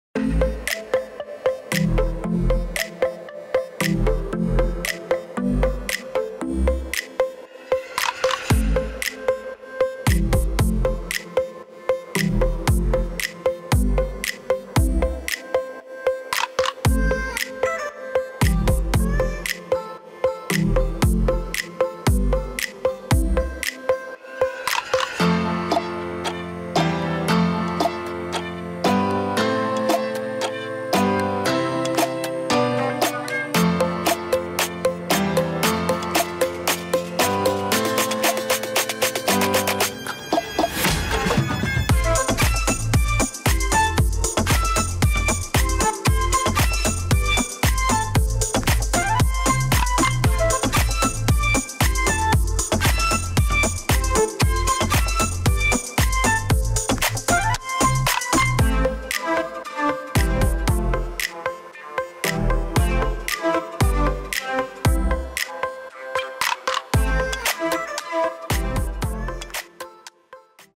Maxi-Cosi | Dana For2 stroller | How to install your baby
Watch this video to learn how to install your baby on your new Maxi-Cosi Dana For2 double stroller.